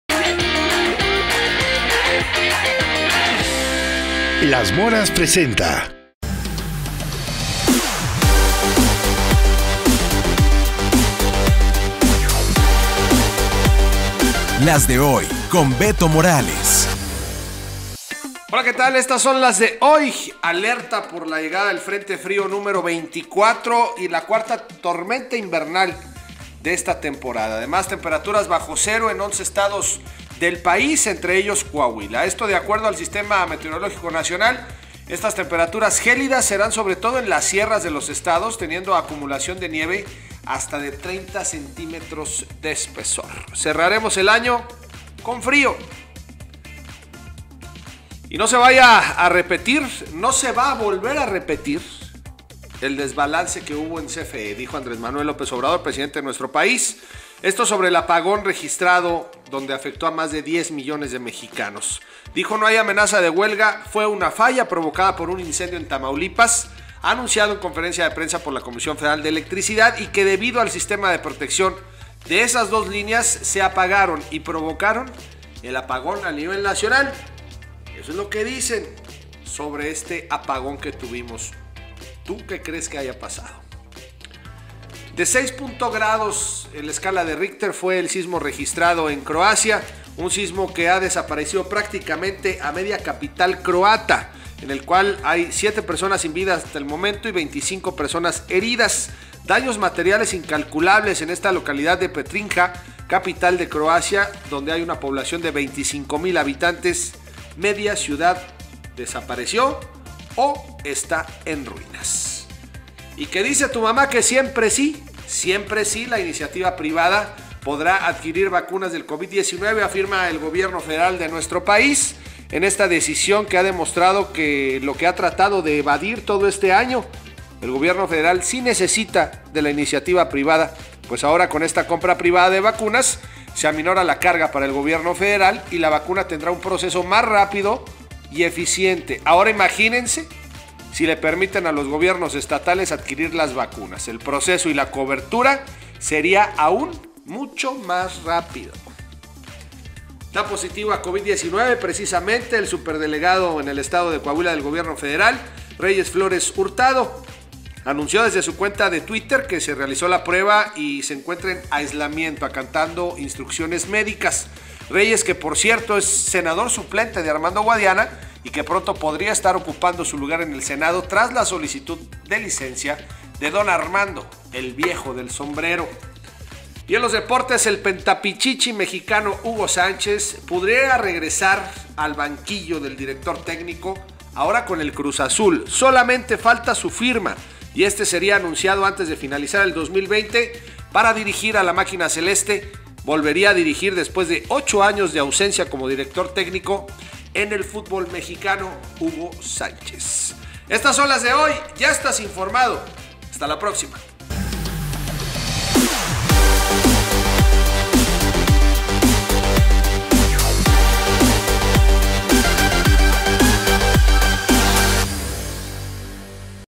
Las de hoy 29 de diciembre 2020
Si te perdiste de la información más relevante del día de hoy martes 29 de diciembre, aquí te dejamos un resumen en LasDeHoy con Beto Morales